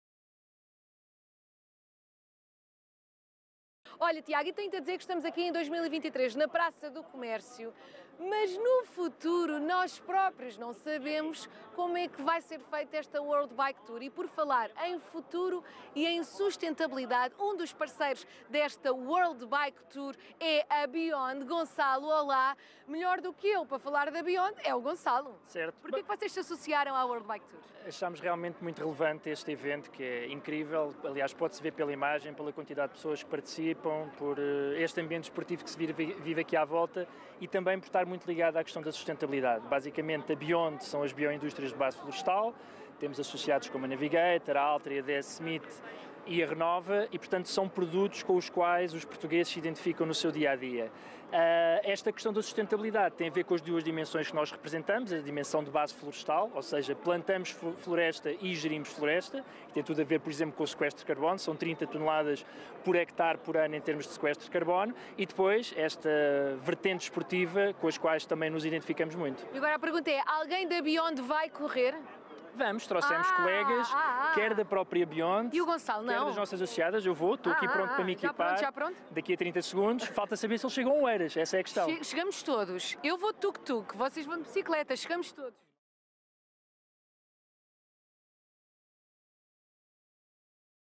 Biond WBT - Entrevista RTP a Gonçalo Almeida Simões
A Biond pedalou com muita fibra no WBT2023 💪🚴‍♀️ e a RTP entrevistou em direto Diretor Geral, Gonçalo Almeida Simões, minutos antes da partida.

Reportagem: RTP